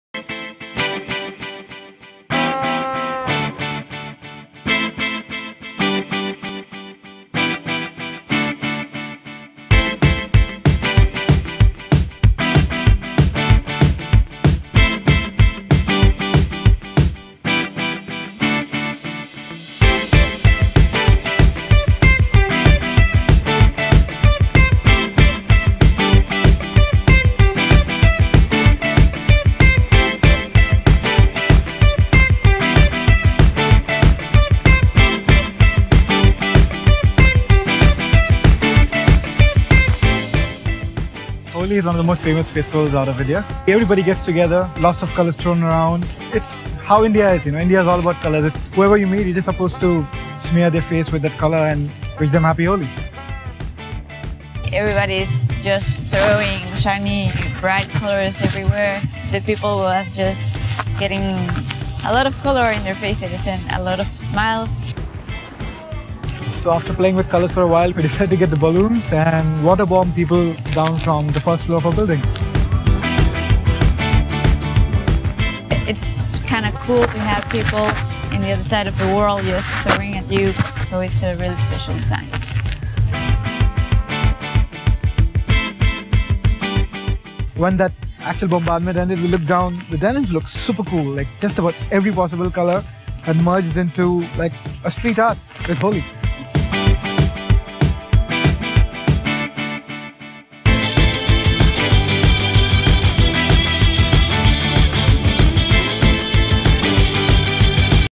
For DENIZEN from Levis - Lorena Garal and Nawed Kahn get color bombed in Jaihpur
Lorena Garal and Nawed Kahn get color bombed in Jaihpur (DENIZEN from Levis). Follow Lorena/ Síguela en Instagram: @LorenaGaral

Lorena Garal, piloto de motocross y el DJ Nawed Khan estan en Jaihpur India para la campaña de DENIZEN from Levis ¨We Are Explorers¨.

En Febrero se festeja una de las fiestas mas grandes del pais, ¨Holli¨, un dia en que todo el pais se llena de color y la gente desconocida se vuelve conocida! Sin duda una gran experiencia!! A compaña a nuestros 2 exploradores en esta gran aventura!